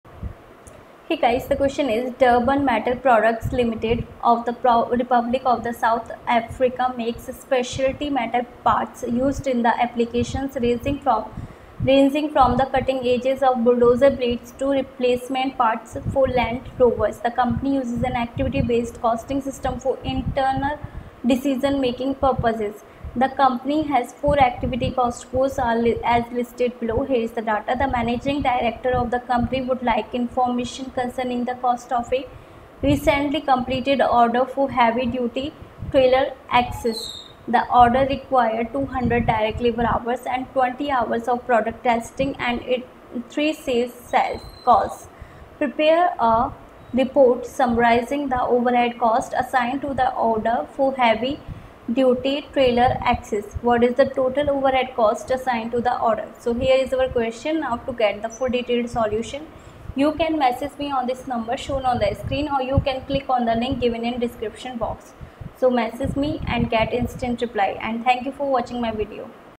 Durban Metal Products, Ltd., of the Republic of South Africa makes specialty metal parts used in
Or

Durban Metal Products, Ltd., of the Republic of South Africa makes specialty metal parts used in applications ranging from the cutting edges of bulldozer blades to replacement parts for Land Rovers. The company uses an activity-based costing system for internal decision-making purposes. The company has four activity cost pools as listed below: The managing director of the company would like information concerning the cost of a recently completed order for heavy-duty trailer axles. The order required 200 direct labor-hours, 20 hours of product testing, and 3 sales calls. Required: Prepare a report summarizing the overhead costs assigned to the order for heavy-duty trailer axles. What is the total overhead cost assigned to the order?